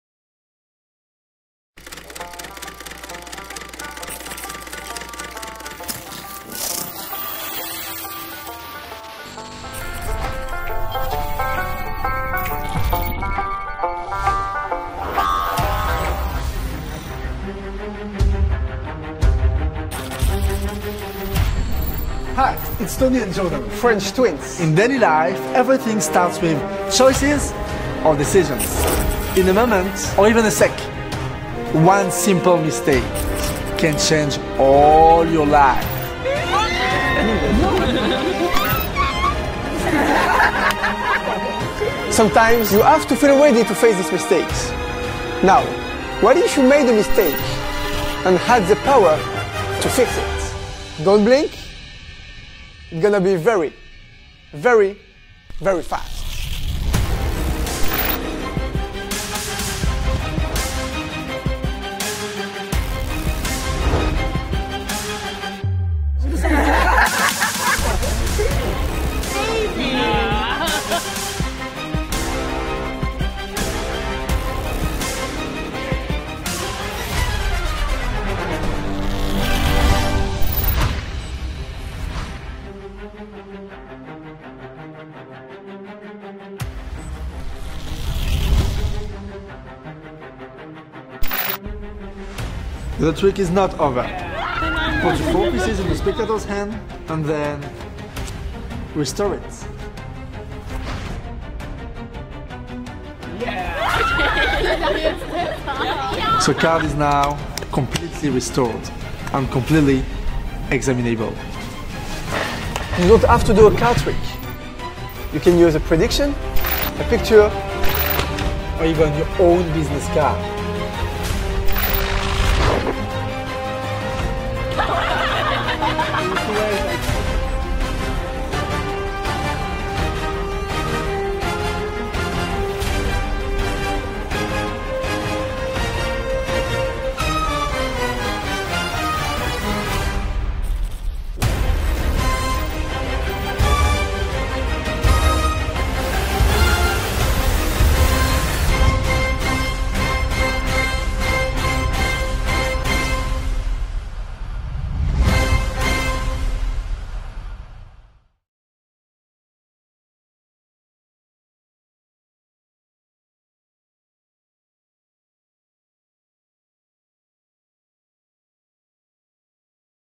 ERROR 404 BY LES FRENCH TWINS - DAYTONA MAGIC
The French TWINS ARE BACK! The French TWINS are known for creating powerful and innovative magic tricks! Visually transform, one by one, 4 torn pieces into the chosen card -- and then restore it in the spectator's hand!